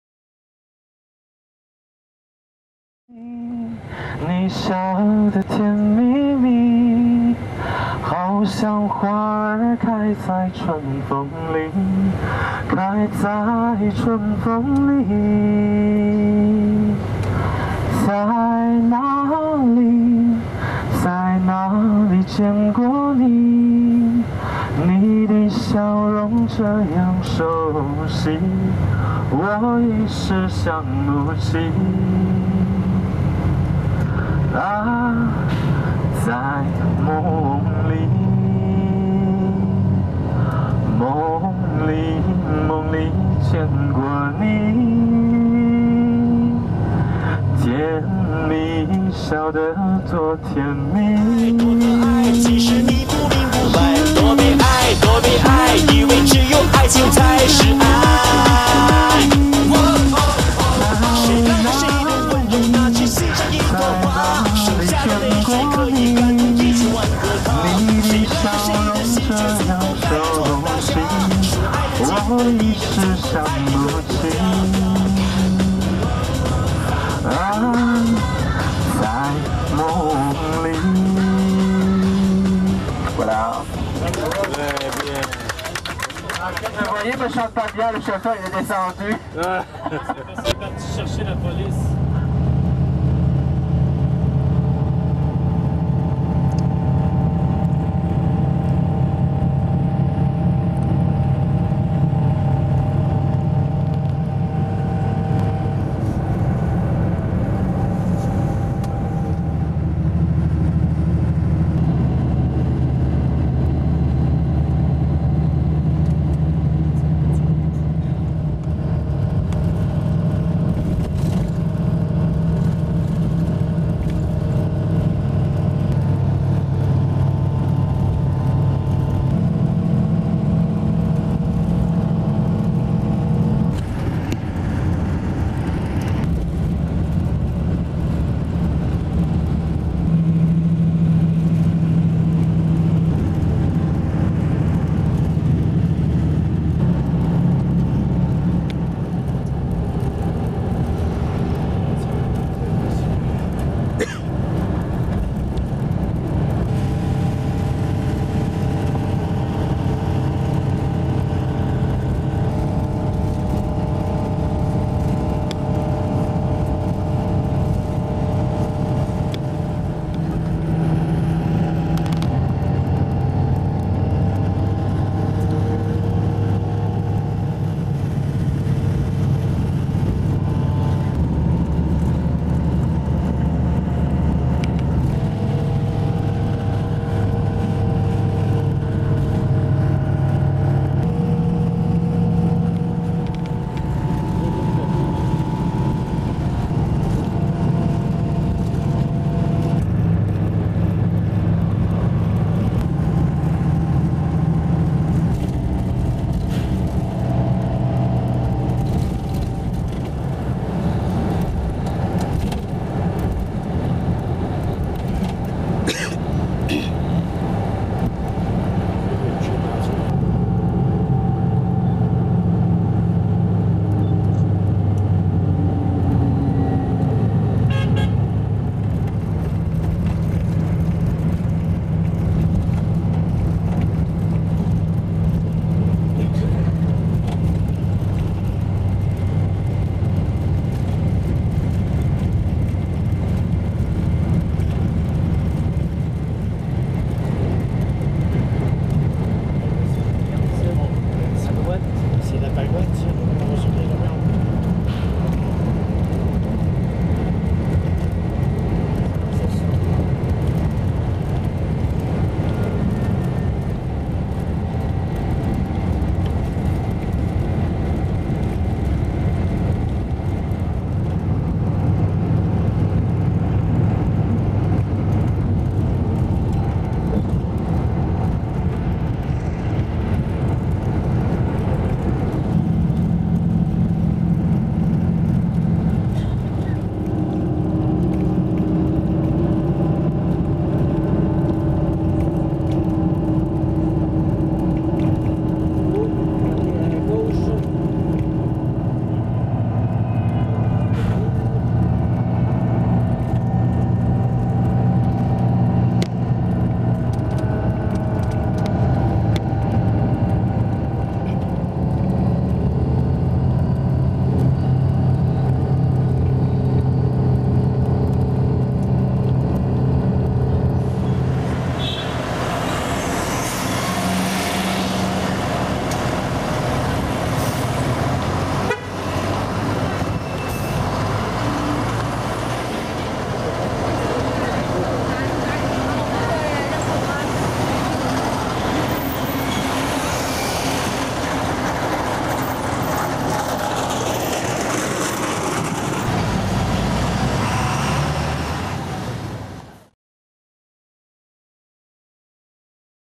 la route de Dali(TY21-03,04) Vietnam
Route périlleuse entre Dien Bien Phu et Dali et la frontière du Yunnan en Chine.